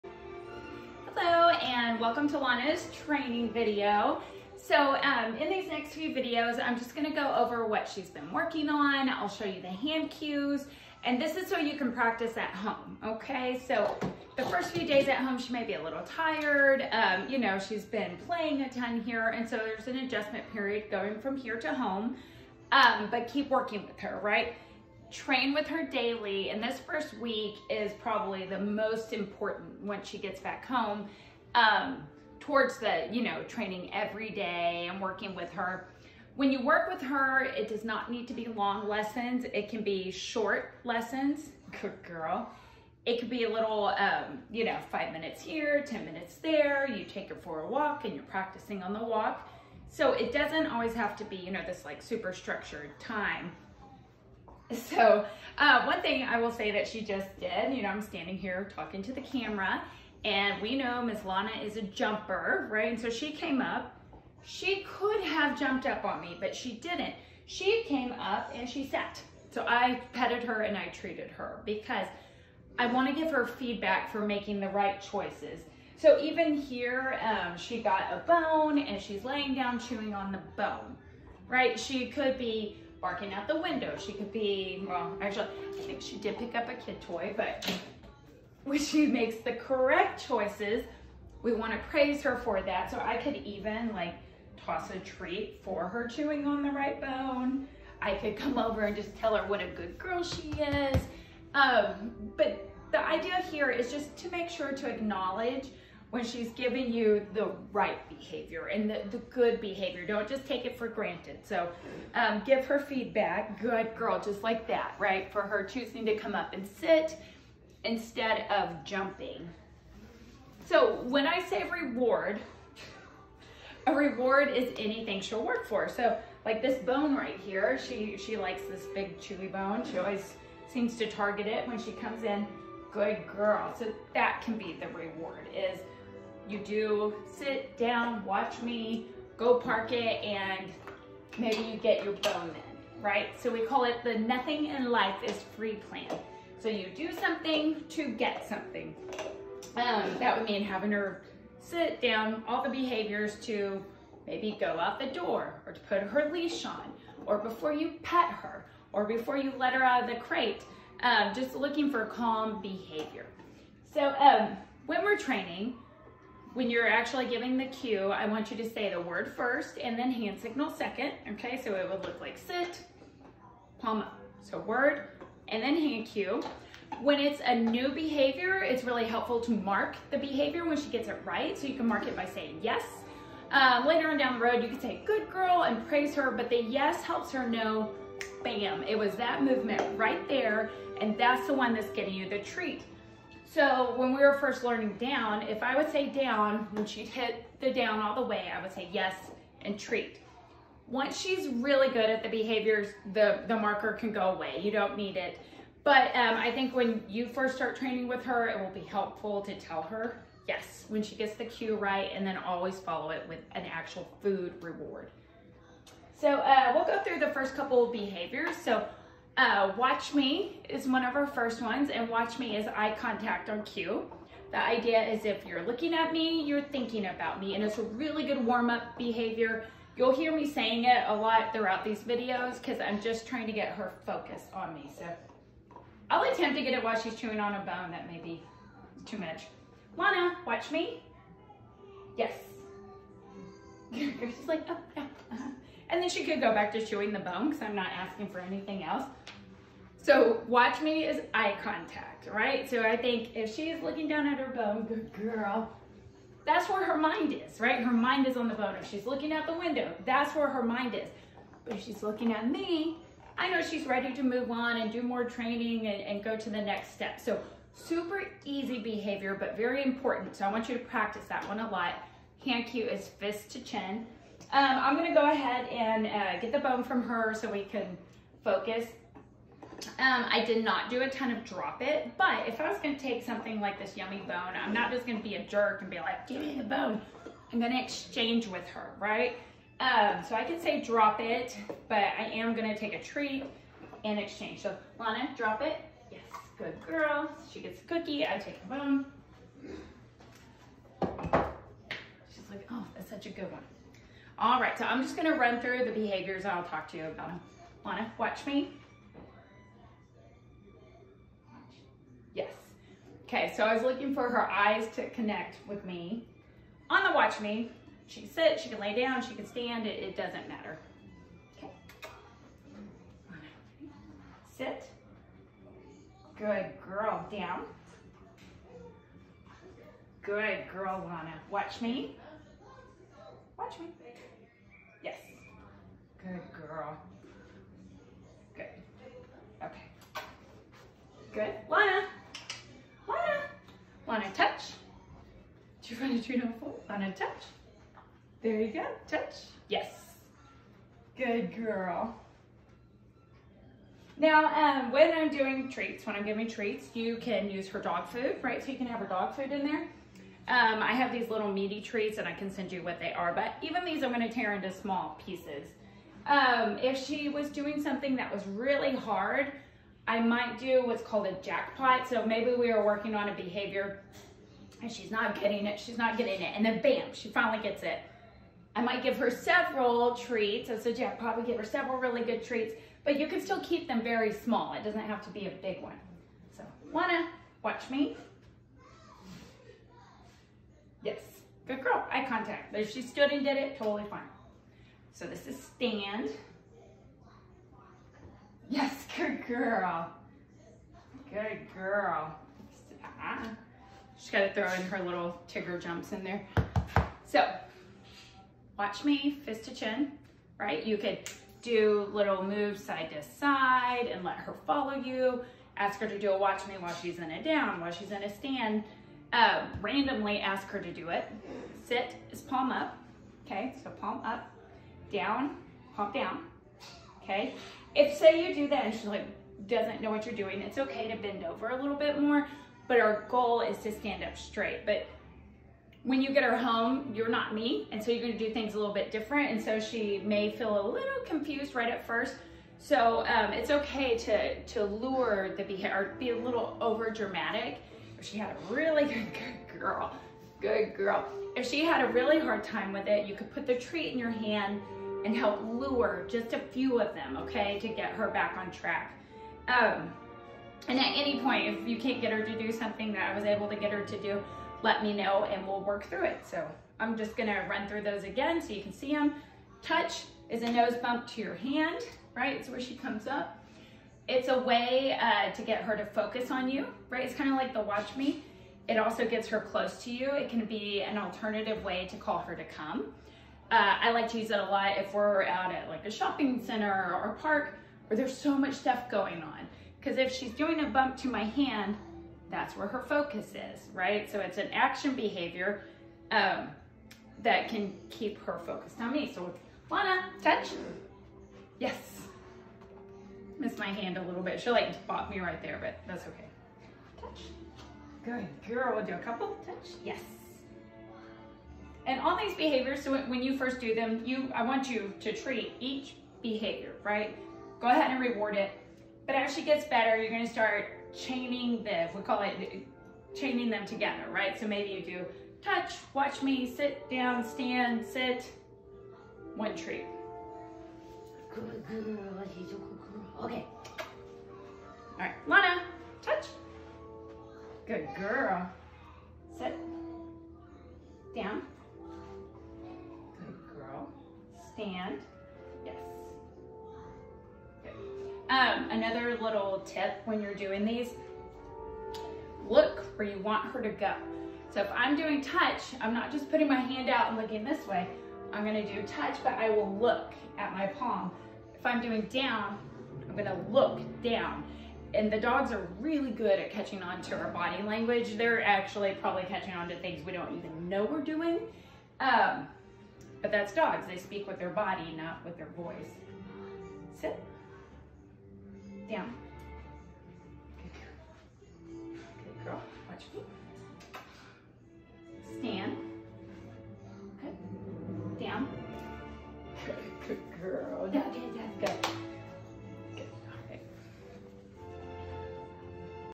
Lana- Intro, Sit, Down, Stand & Touch
Lana- adolescent standard poodle